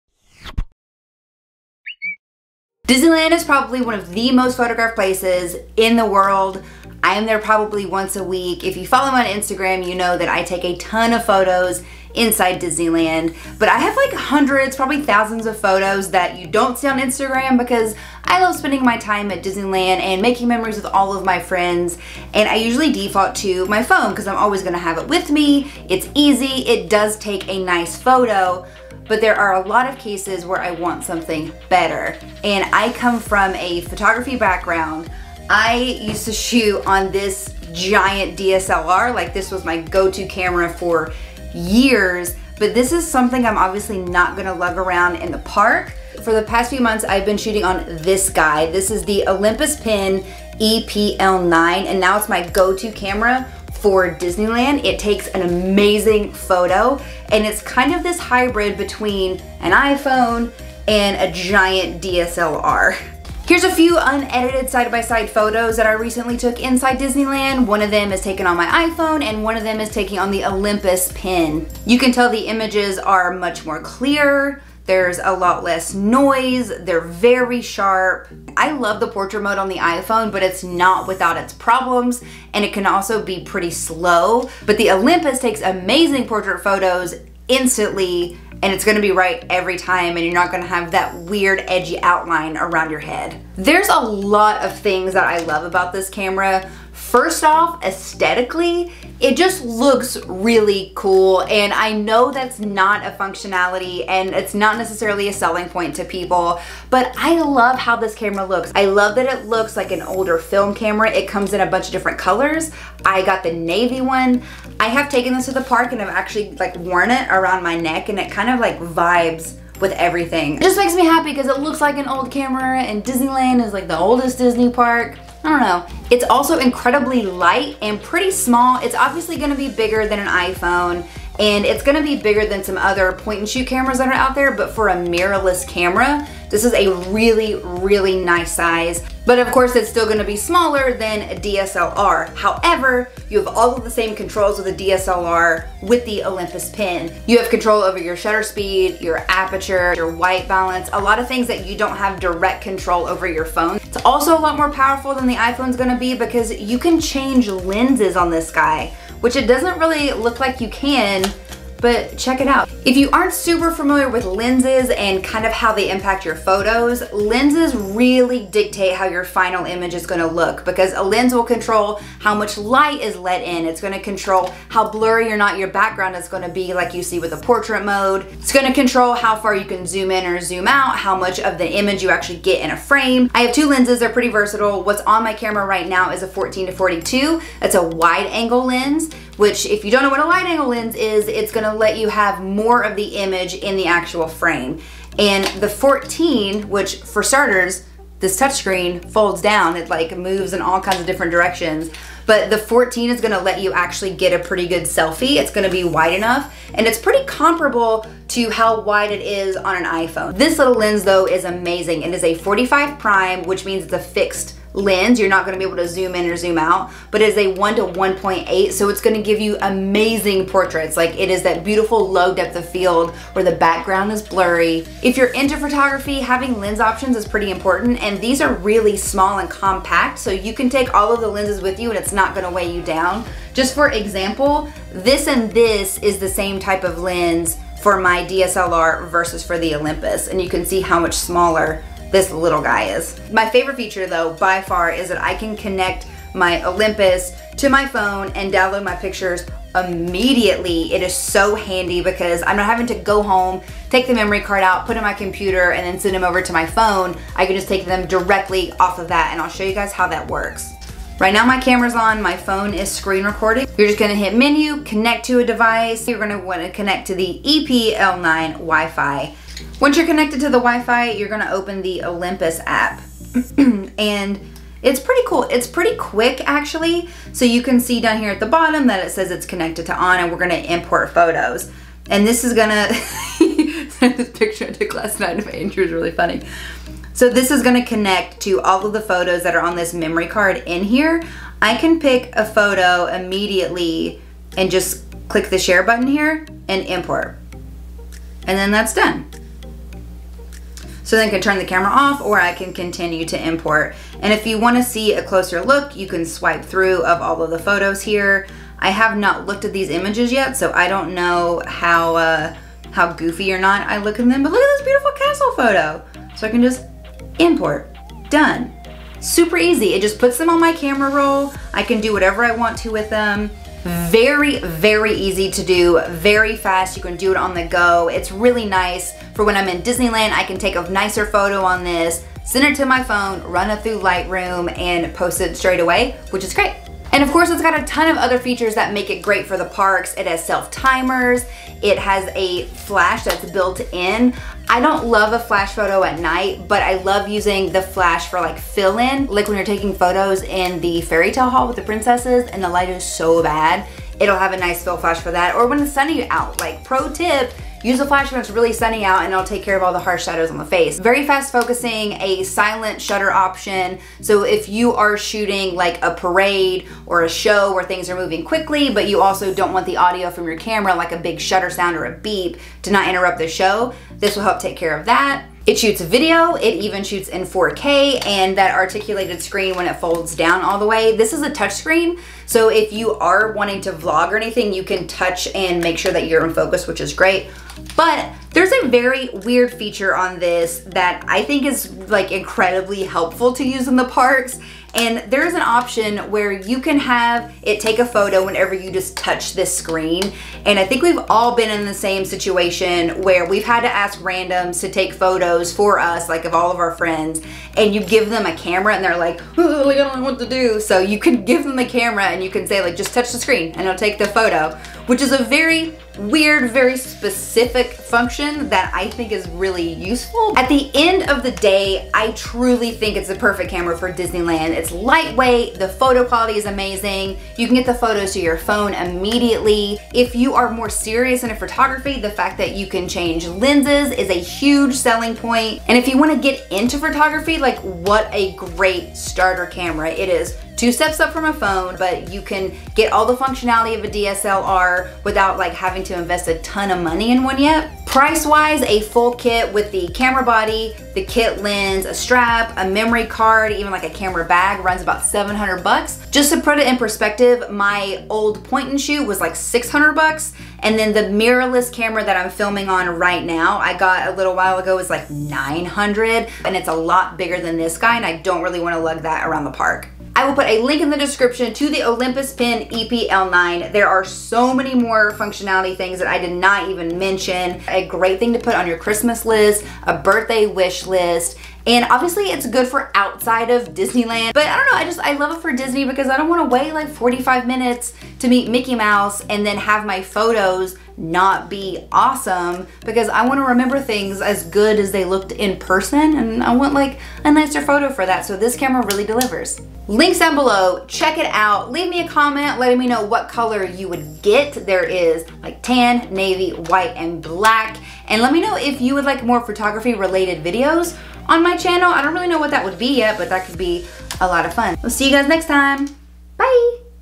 The BEST Camera for Disneyland!
Thanks to Olympus for letting me check out the Pen E-PL9! After using it for a few months, I think it's the best camera to take on your Disney park vacation!